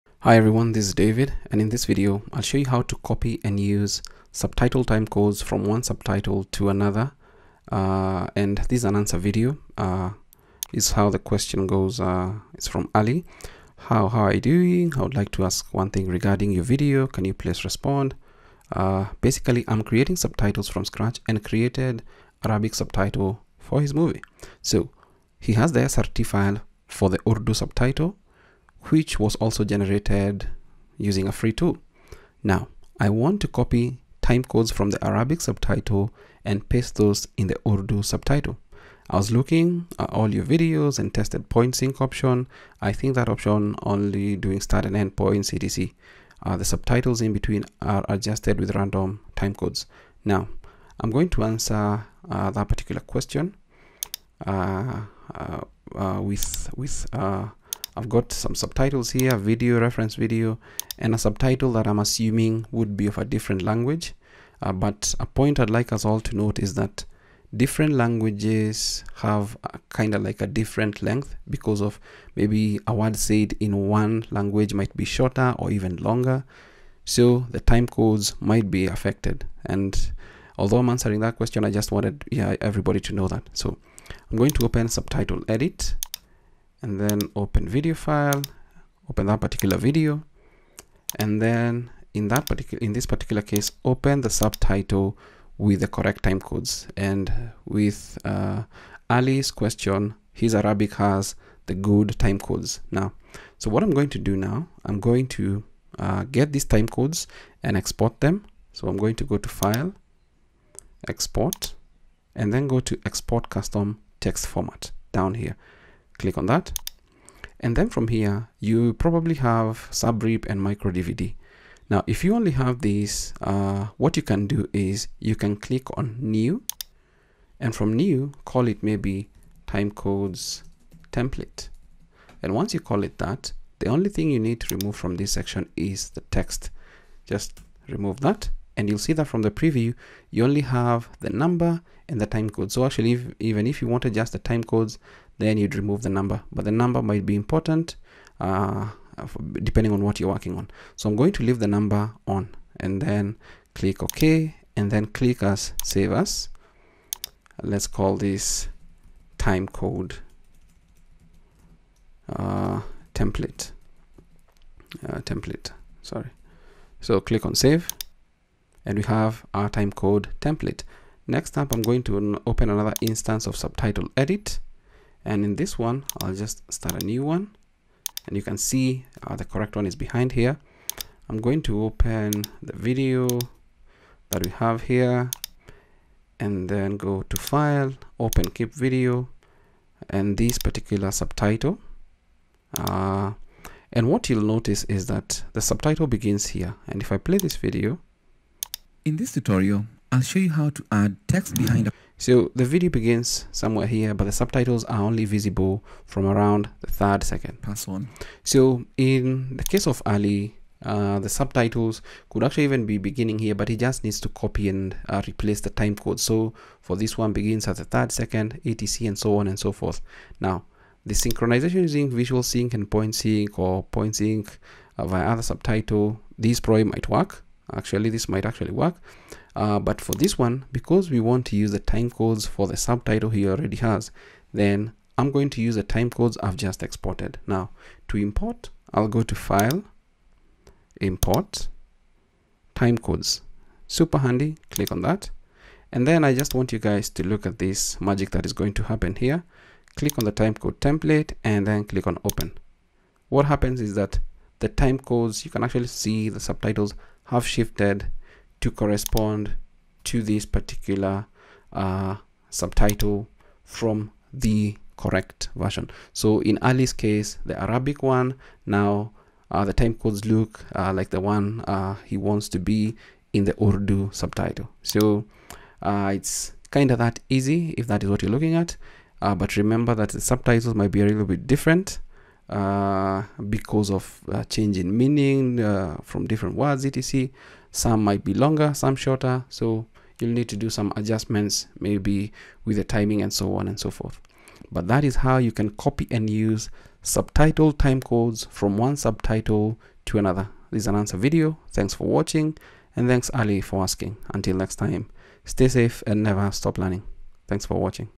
How to Copy and Use Subtitle Timecodes Between 2 Subtitles in Subtitle Edit - Answer Video
In this video, I'll show you How to Copy and Use Subtitle Timecodes from one Subtitle to Another in Subtitle Edit